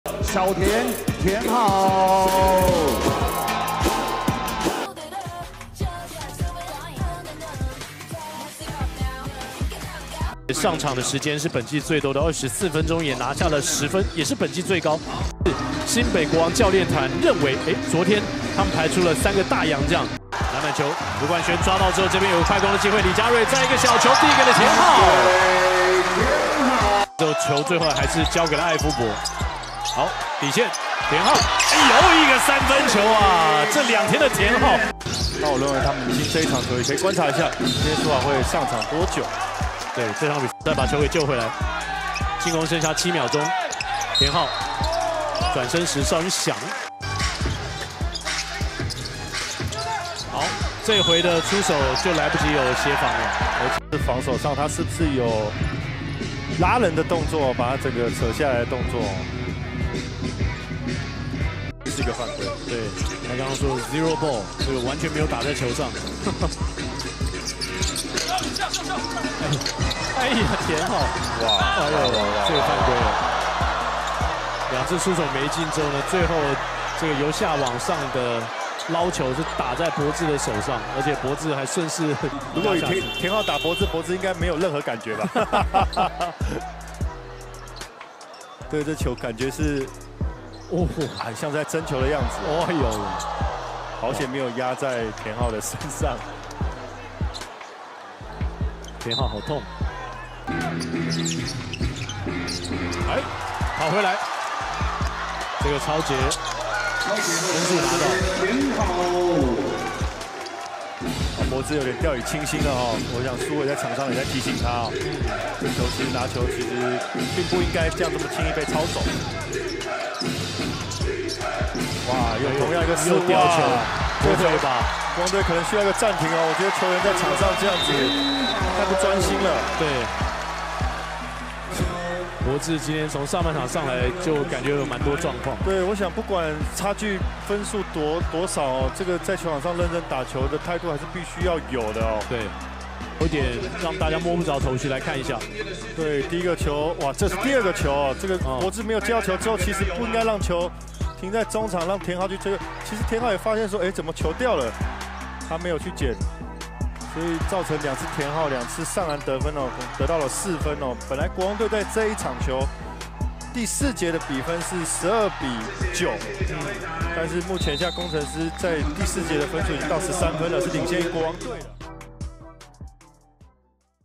田浩 頂替高國豪先發上陣把握機會 連續輕鬆抄截王柏智讓林書緯爆氣 PLG 新北國王 vs 新竹御頂攻城獅
歡迎加入頻道會員支持我們更新影片